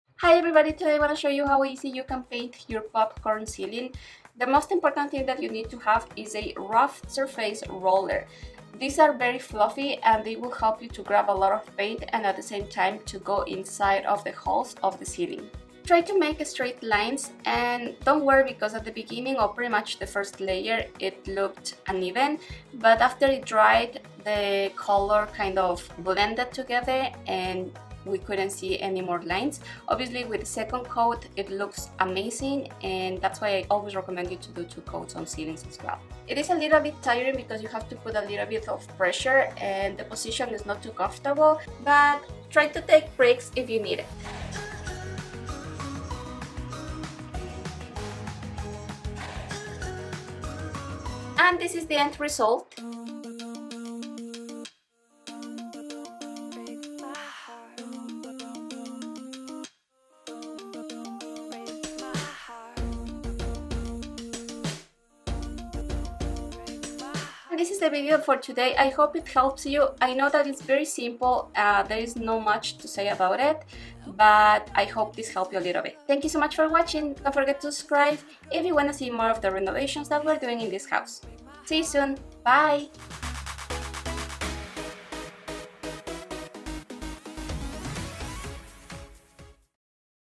HOW TO PAINT POPCORN CEILING
I want  to show you how to paint the ceiling
A Popcorn Ceiling, also known as a stipple ceiling, a stucco ceiling or formally an acoustic ceiling, is a ceiling with a certain spray-on or paint-on treatment.